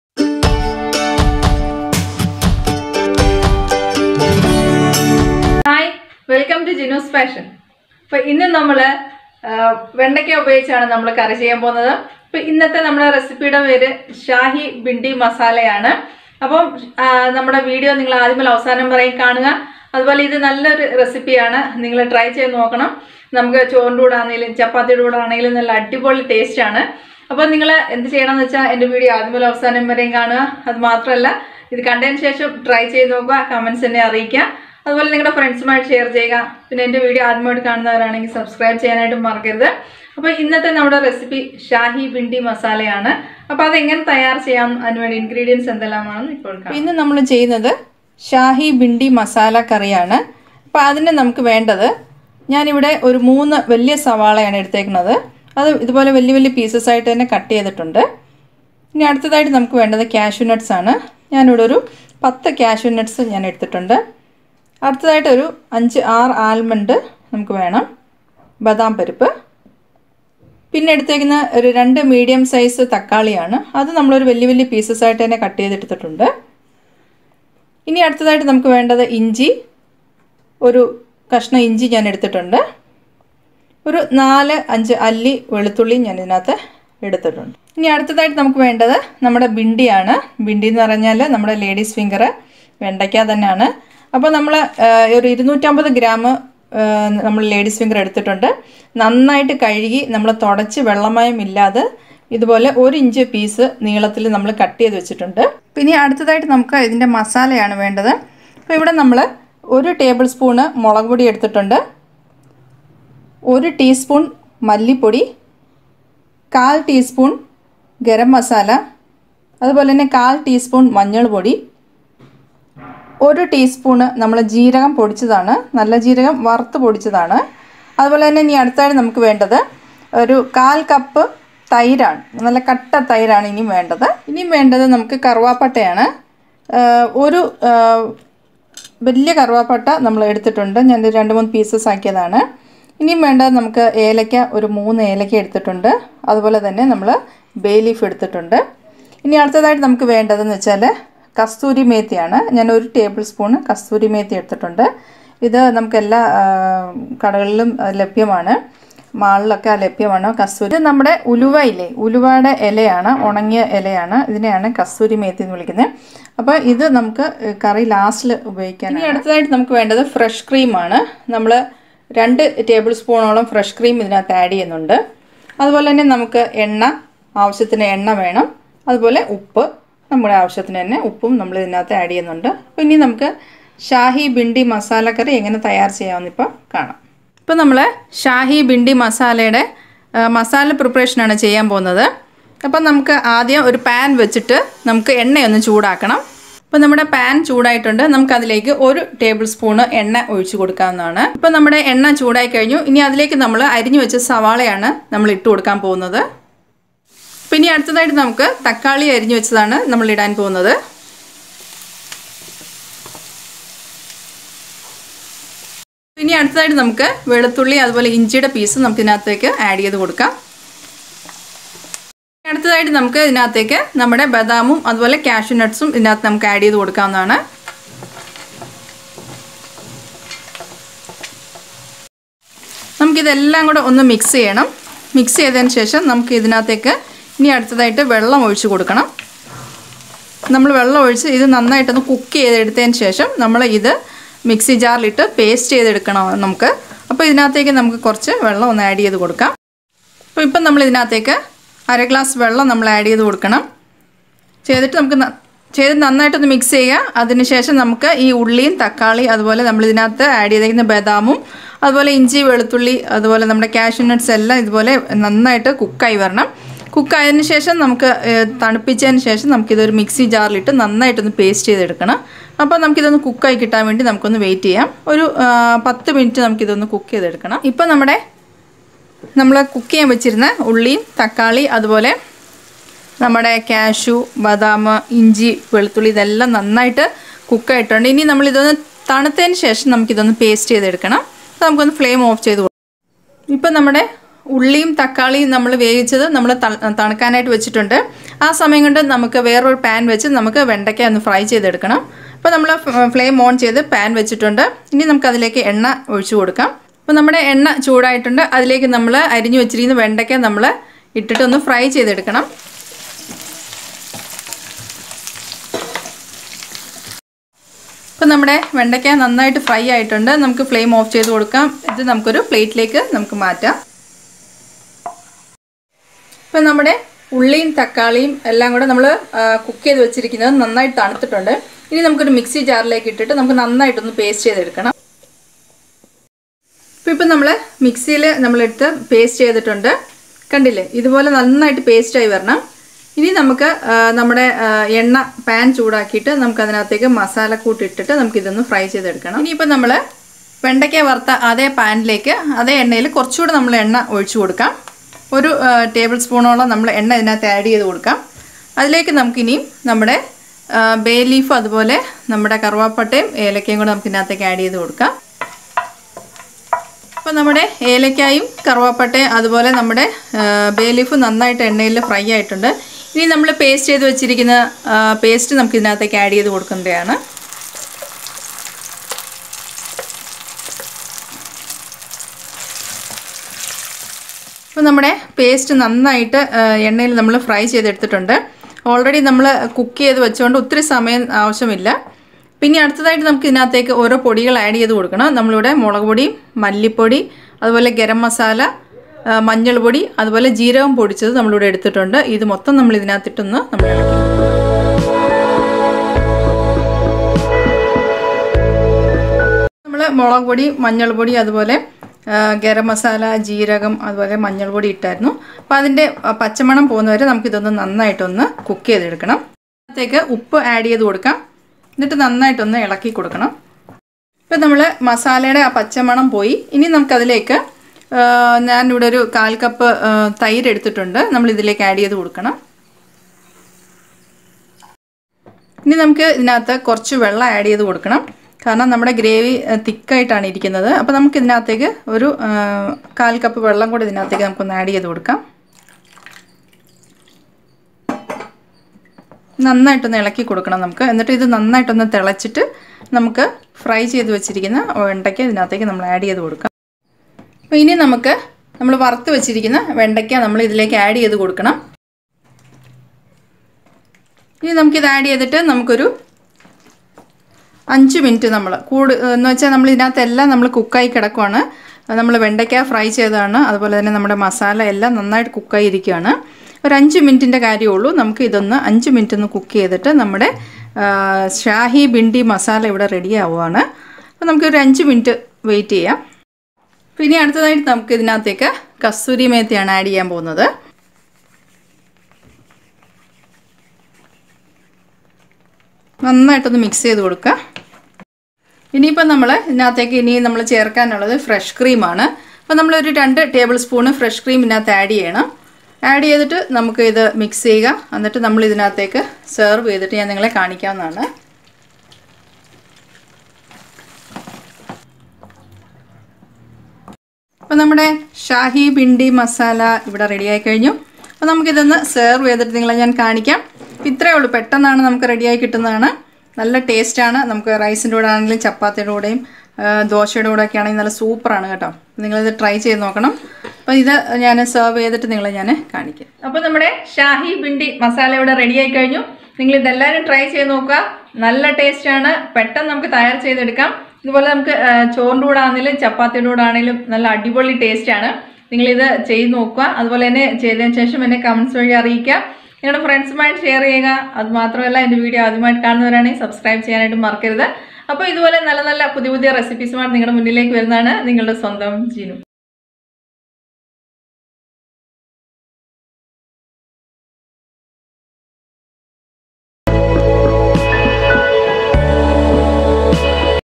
Shahi Bhindi Masala Gravy | Okra in a creamy | shahi bhindi sabzi
Shahi Bhindi Recipe in Cashew Nut Gravy  Bhindi curry recipe, this Masala curry, is rich in all flavors of Nuts , Vegetables, Fresh Cream etc,  Bhindi or okra dishes turn out to be an all time favorite of India, you will love this curry, a recommended  Curry for you and your family